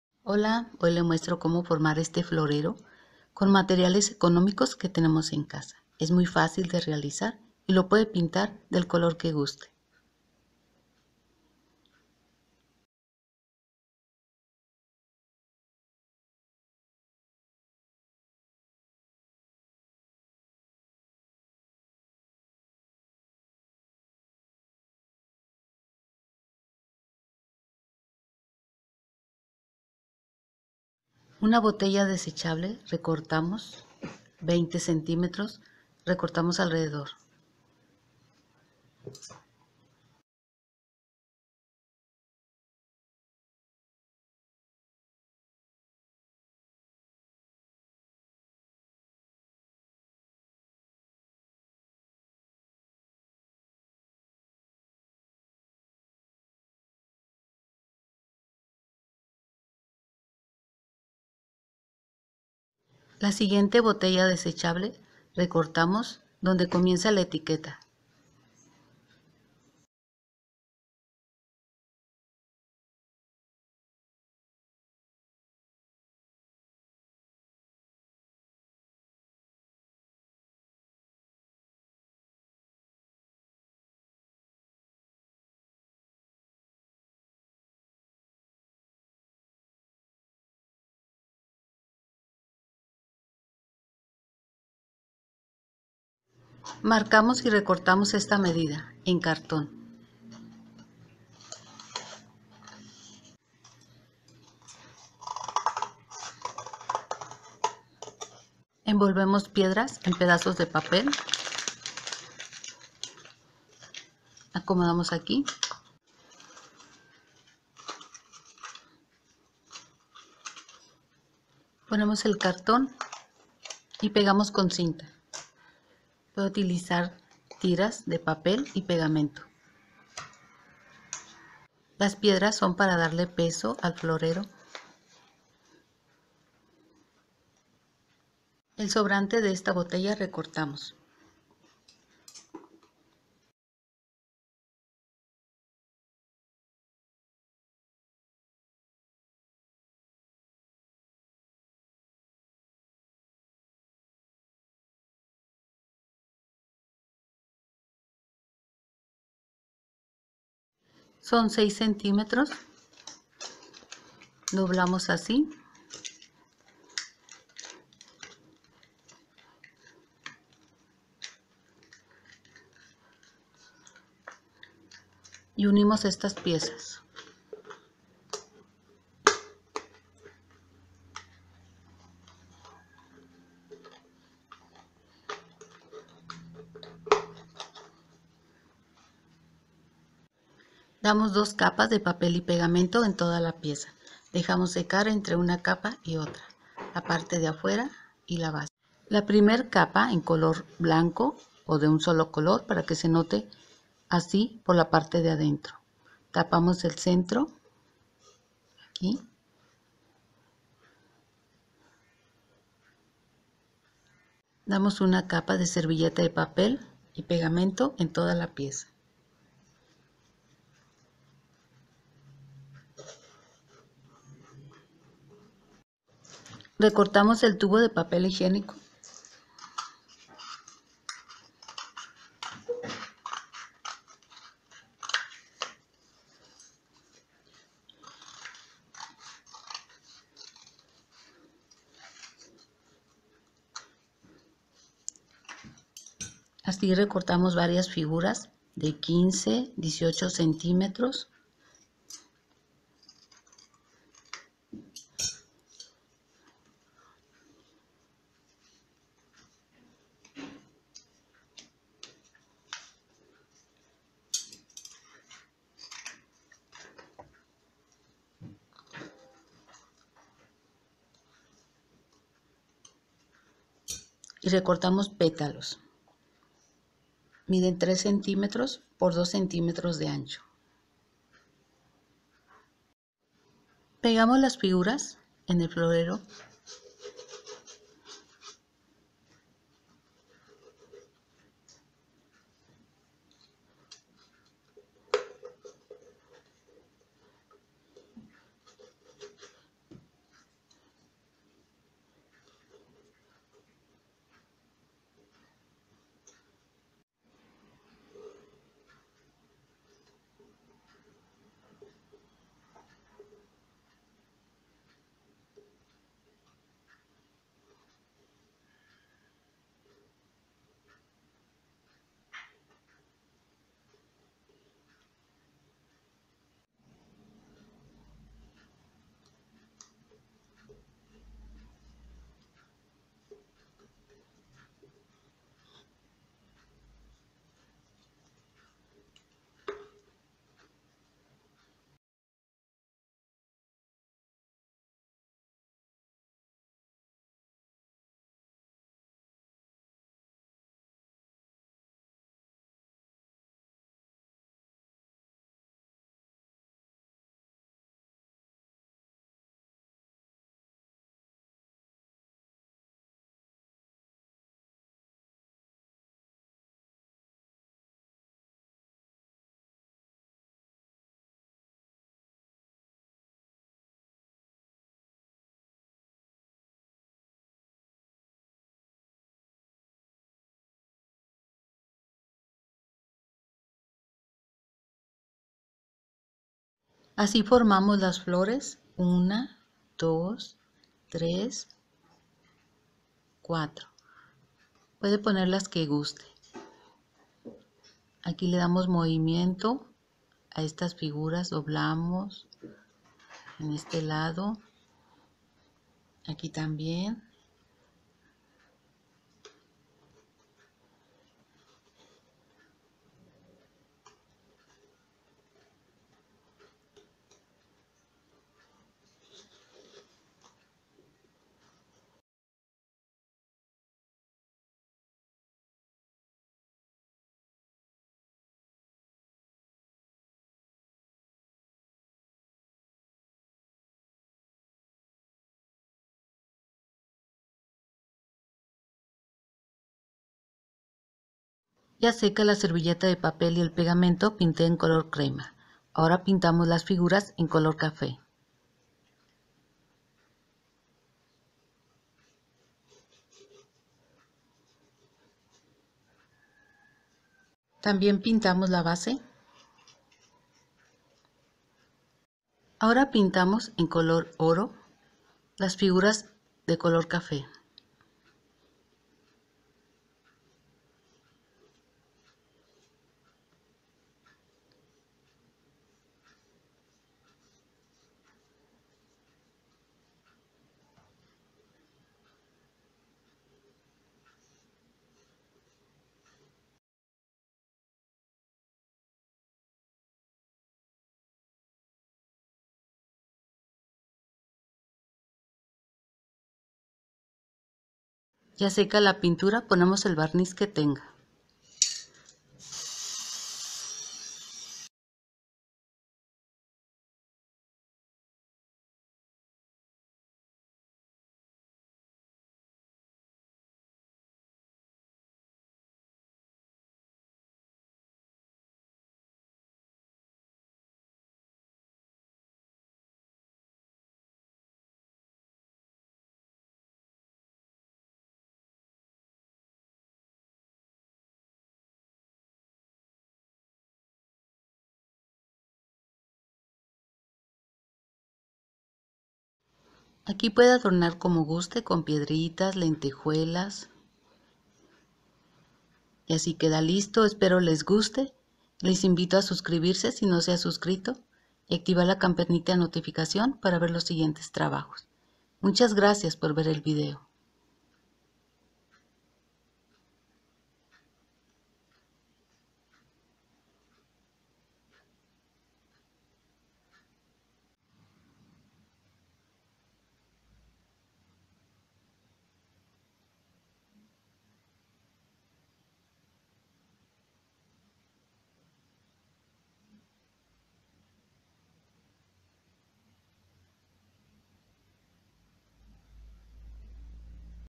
con botella desechable y tubos del papel higienico!
facil y economica con materiales que tenemos en casa  MATERIALES  carton, 2 botellas desechables , pegamento, piedras pequeñas ,pintura ,servilleta de papel o papel higienico,